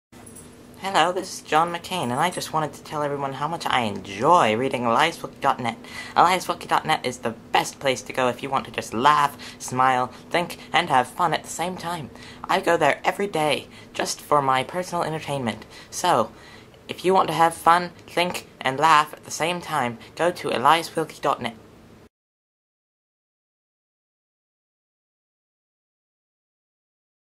John McCain endorses eliaswilkey.net
john mccain gives his opinion on a highly popular humor website written by tkraz907 himself.